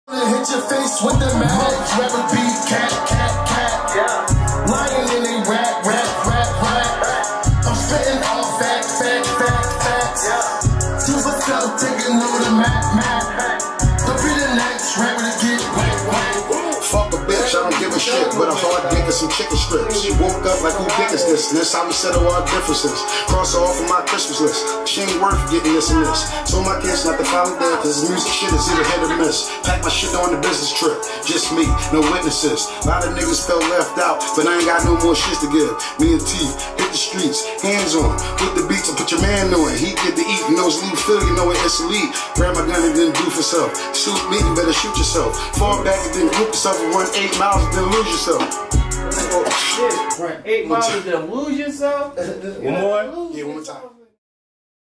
Teefy Bey & E Ness in the studio
Teefy bey from D4S and Dreamchasers links with rap battle legend E Ness from the original Making The Band!